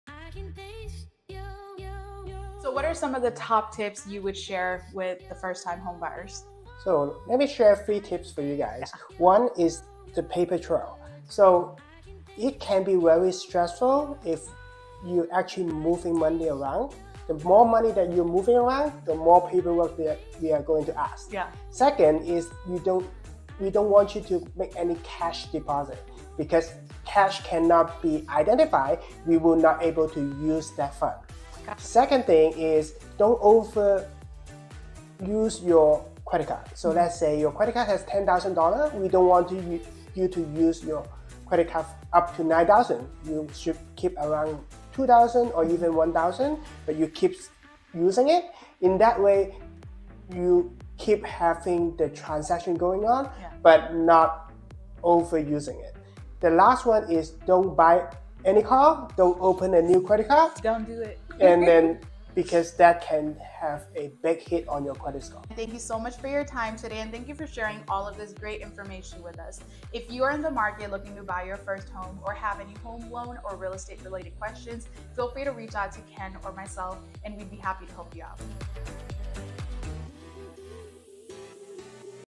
Top Tips for First Time Home Buyers
If you are starting to prepare to purchase your first home, these 3 tips from leading professionals Ruby Sangha with Coldwell Banker Realty and Kenneth Lee with Guaranteed Rate Affinity are a must see for you!